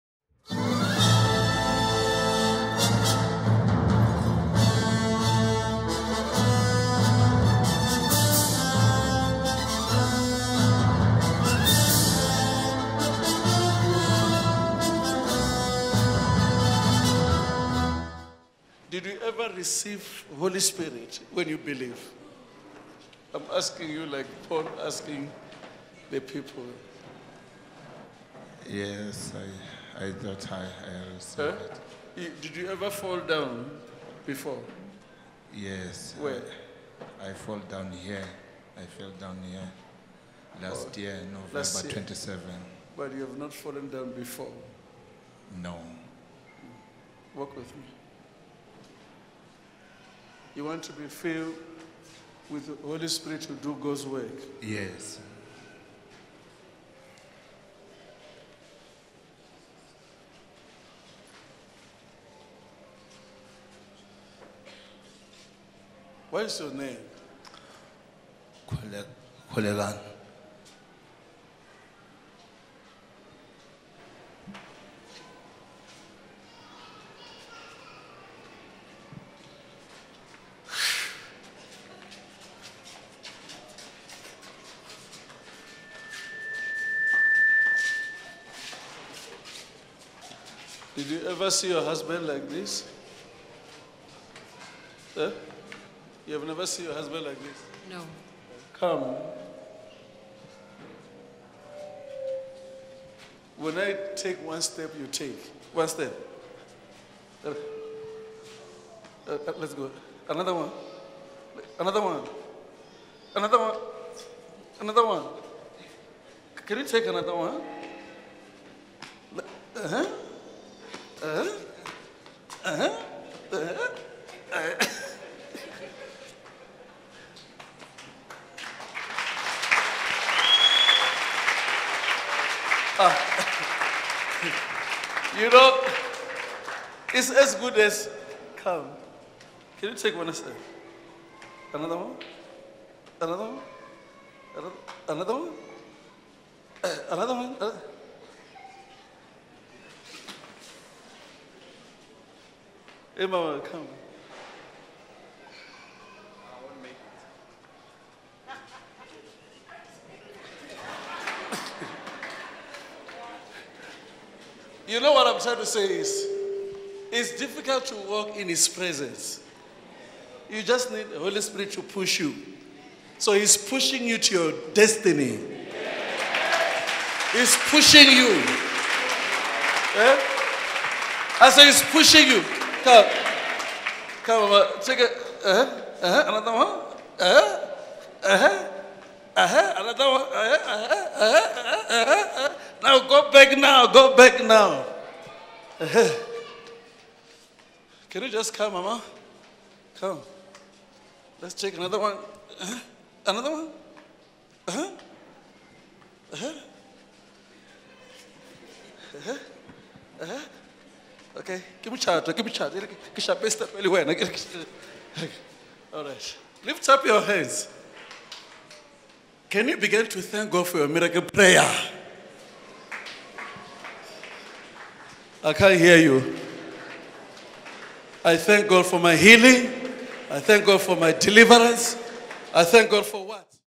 YOU NEED HOLY SPIRIT to walk in GOD'S PRESENCE ll Demonstration of Power
See how Apostle JB Makananisa is used to fill a fellow Servant of God with the Holy Spirit